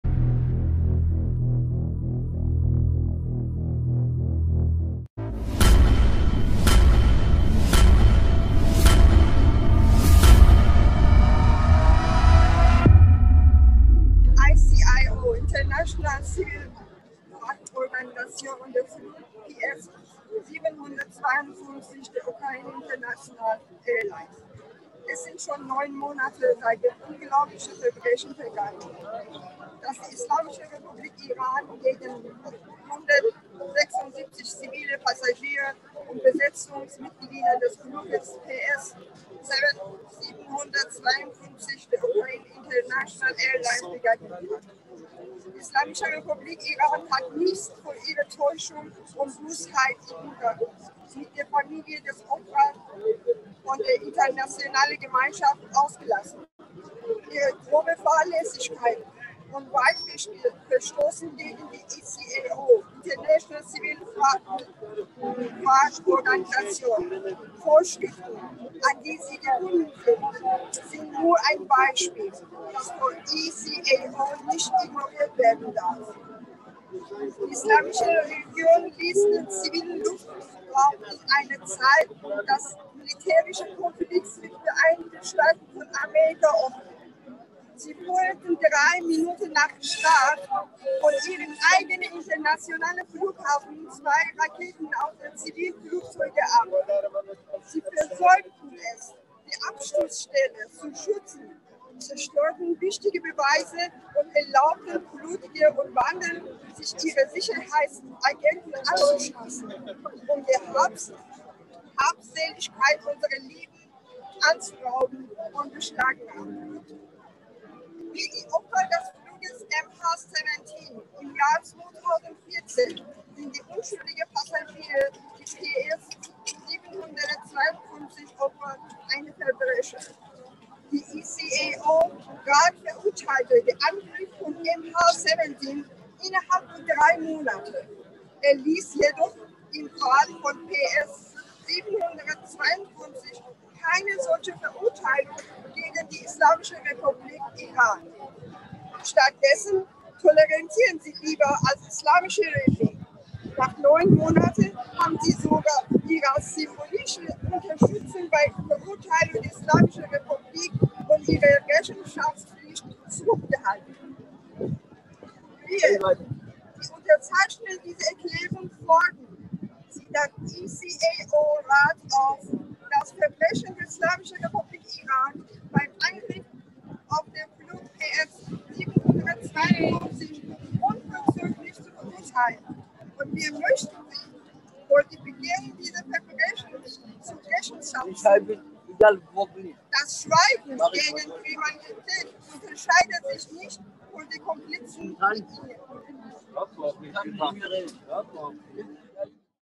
JusticeRally - Nasrin Jalali live in Frankfurt
Nasrin Jalali (Human Rights Activist) speaking live in Frankfurt JusticeRally - MyZeil

Speech Text in German:
ICAO(Internationale Zivilluftfahrtorganisation) und der Flug PS752 der Ukraine International Airlin!
Es sind schon neun Monate seit dem unglaublichen Verbrechen vergangen, das die Islamische Republik Iran gegen 176 zivile Passagiere und Besatzungsmitglieder des Fluges PS752 der Ukraine International Airlines begangen hat.
 Die Islamische Republik Iran hat nichts von ihrer Täuschung und Bosheit im Umgang mit den Familien des Opfers und der internationalen Gemeinschaft ausgelassen.  Ihre grobe Fahrlässigkeit und weitreichende Verstöße gegen die ICAO(Internationale Zivilluftfahrtorganisation)-Vorschriften, an die sie gebunden sind, sind nur ein Beispiel, das von der ICAO nicht ignoriert werden darf.
 Die iranische Regierung ließ den zivilen Luftraum in einer Zeit des militärischen Konflikts mit den Vereinigten Staaten von Amerika offen.  Sie feuerten drei Minuten nach dem Start von ihrem eigenen internationalen Flughafen zwei Raketen auf ein Zivilflugzeug ab.  Sie versäumten es, die Absturzstelle zu schützen, zerstörten wichtige Beweise und erlaubten Plünderern und Vandalen, sich ihren Sicherheitsagenten anzuschließen, um die Habseligkeiten unserer Lieben auszurauben und zu beschlagnahmen.
Wie die Opfer des Fluges MH17 im Jahr 2014 sind die unschuldigen Passagiere der PS 752 Opfer eines Verbrechens.  Der ICAO-Rat verurteilte den Angriff auf MH17 innerhalb von drei Monaten, erließ jedoch im Fall von PS 752 keine solche Verurteilung gegen die Islamische Republik Iran.  Stattdessen tolerierten sie lieber das iranische Regime.
Nach neun Monaten haben Sie sogar Ihre symbolische Unterstützung bei der Verurteilung der Islamischen Republik und ihrer Rechenschaftspflicht zurückgehalten.
 Wir, die Unterzeichner dieser Erklärung, fordern Sie den ICAO-Rat auf, das Verbrechen der Islamischen Republik Iran beim Angriff auf den Flug PS 752 unverzüglich zu verurteilen, und wir möchten sie für die Begehung dieses Verbrechens zur Rechenschaft ziehen.  Das Schweigen gegen Kriminelle unterscheidet sich nicht von der Komplizenschaft mit ihnen.